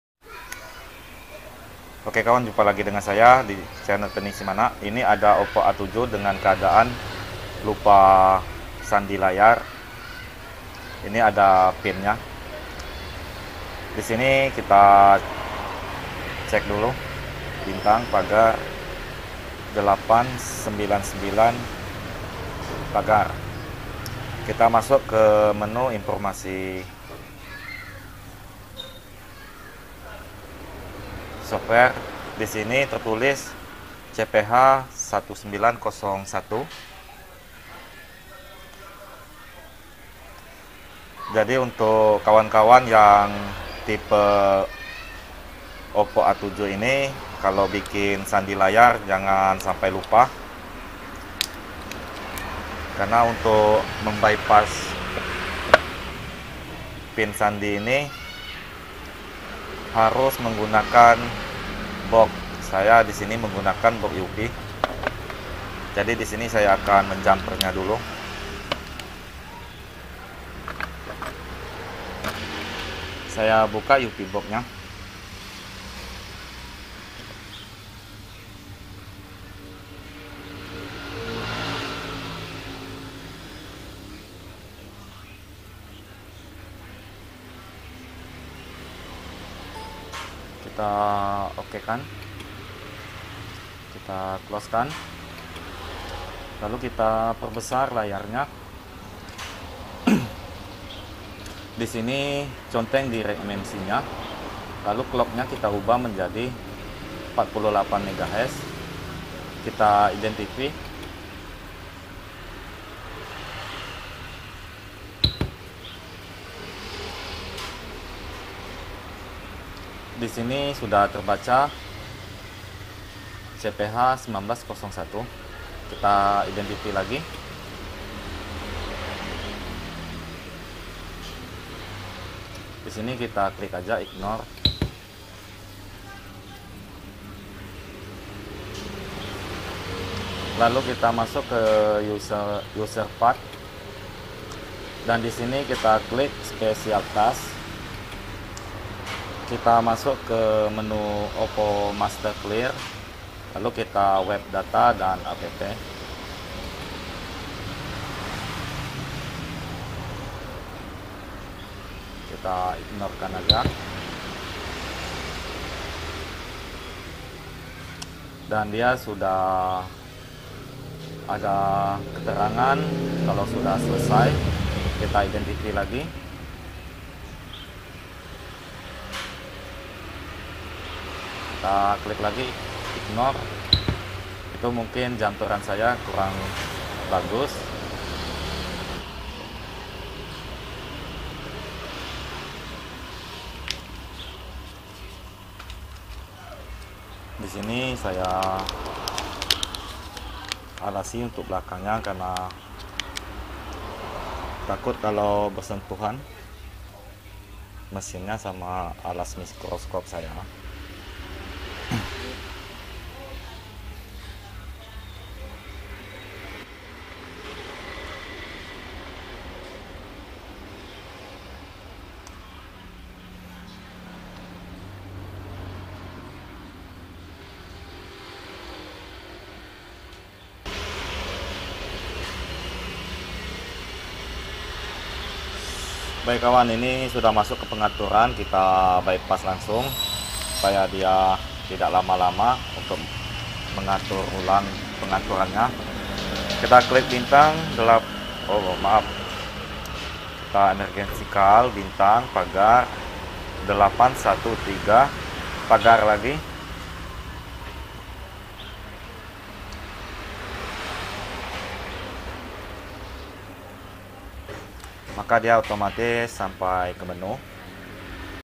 SERVIS OPPO A7 LUPA SANDI LAYAR
cara mengatasi Oppo A7 lupa sandi layar menggunakan ufi box